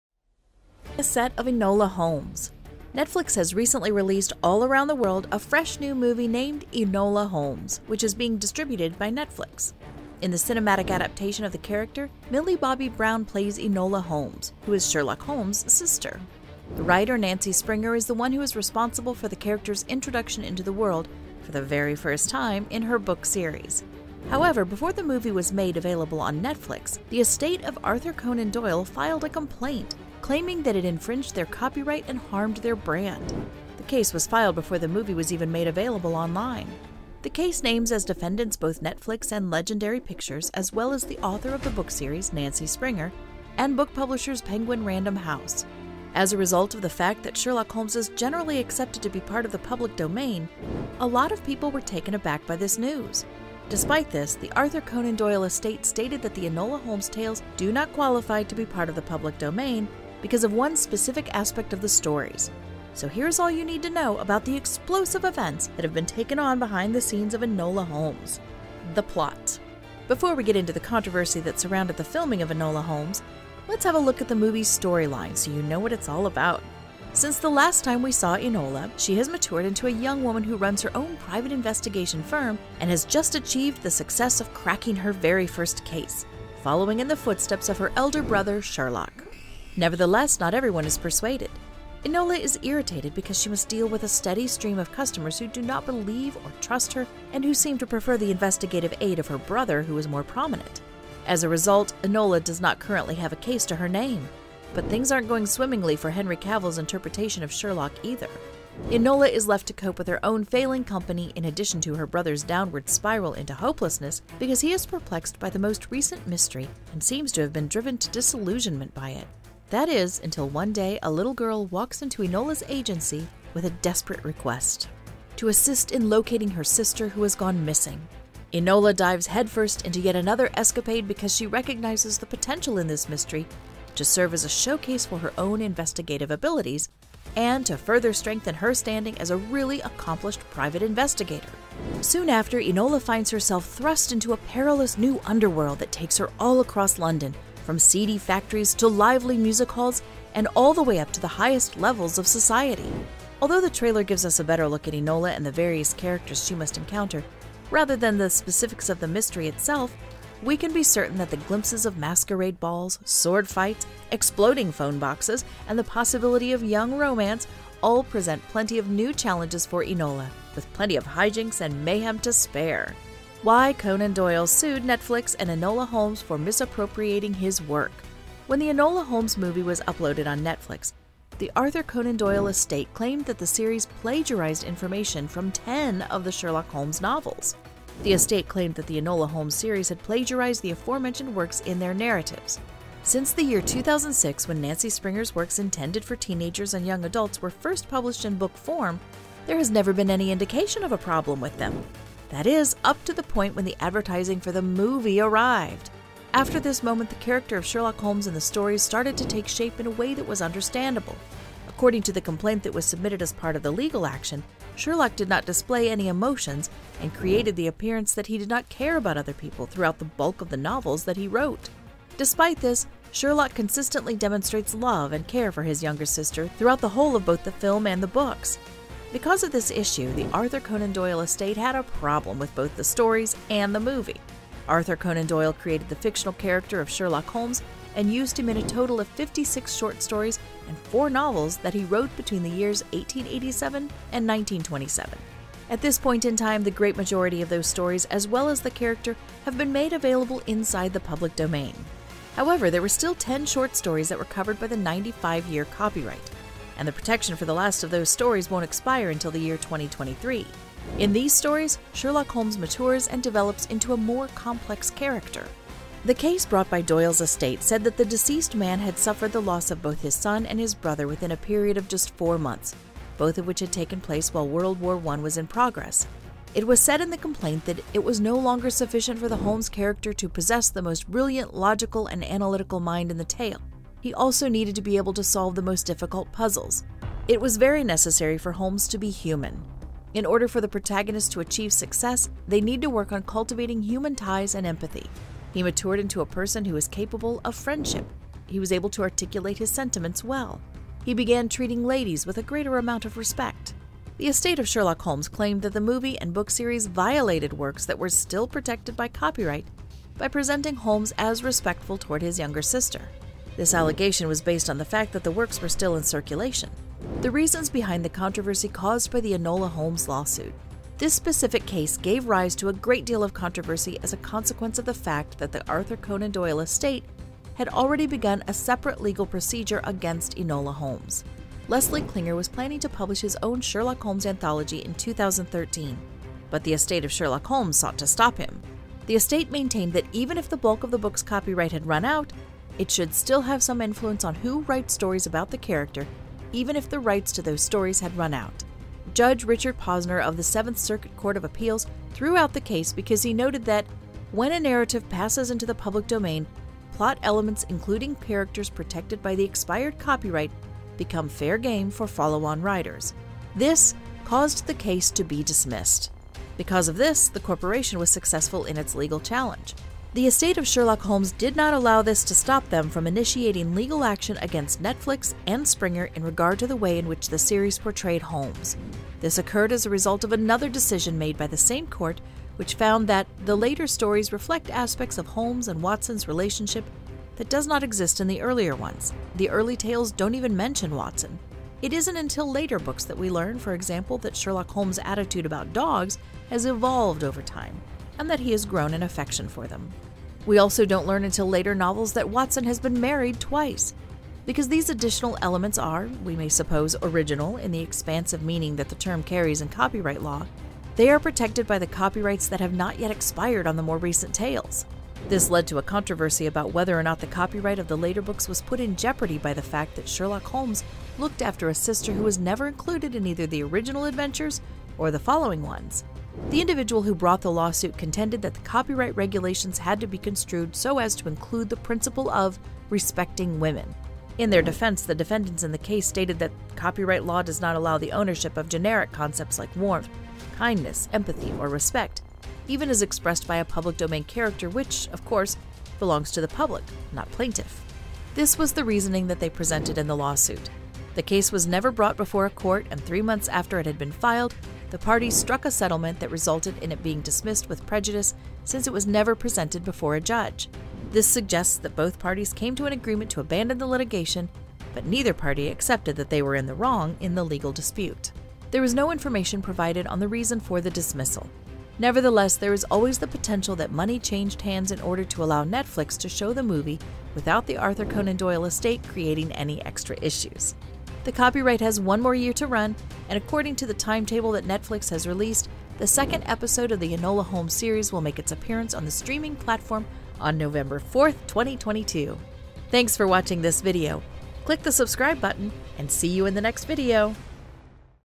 Behind The Set Of Enola Holmes
Netflix has recently released all around the world a fresh new movie named "Enola Holmes," which is being distributed by Netflix. In the cinematic adaption of the character, Millie Bobby Brown plays Enola Holmes, who is Sherlock Holmes' sister. The writer Nancy Springer is the one who is responsible for the character's introduction into the world for the very first time in her book series.

So, here is all you need to know about the explosive events that have been taken on behind the scenes of Enola Holmes.